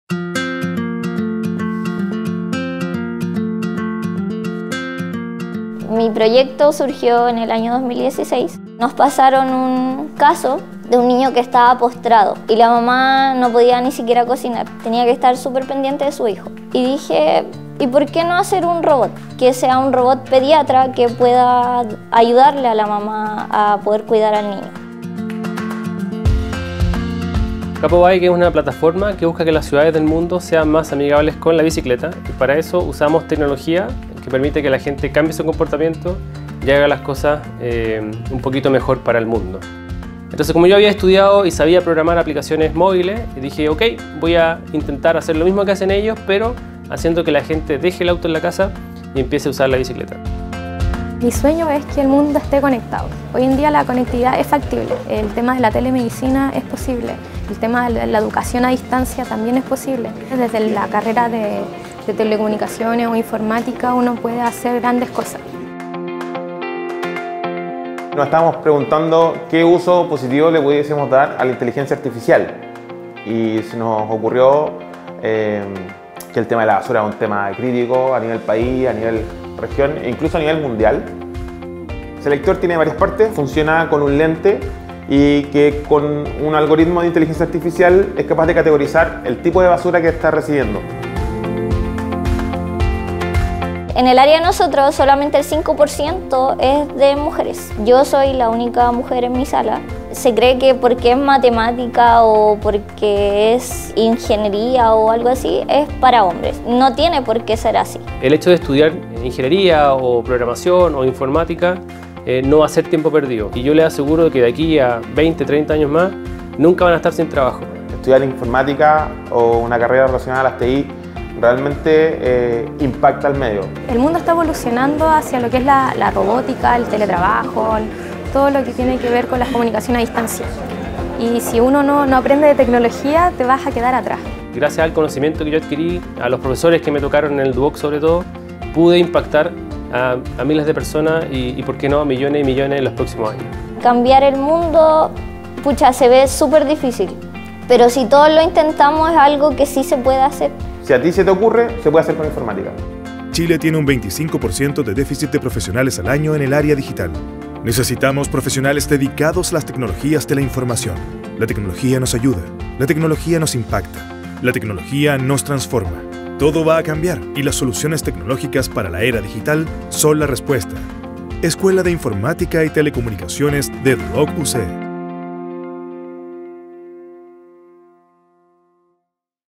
VIDEO Escuela de Informática y Telecomunicaciones de Duoc UC
La Escuela de Informática y Telecomunicaciones de Duoc UC busca formar personas, protagonistas de los cambios, que a través de sus conocimientos sean capaces de aportar y transformar a la sociedad de hoy.
“La tecnología nos ayuda, la tecnología nos impacta, la tecnología nos transforma. Todo va a cambiar y la soluciones tecnológicas para la era digital son la respuestas”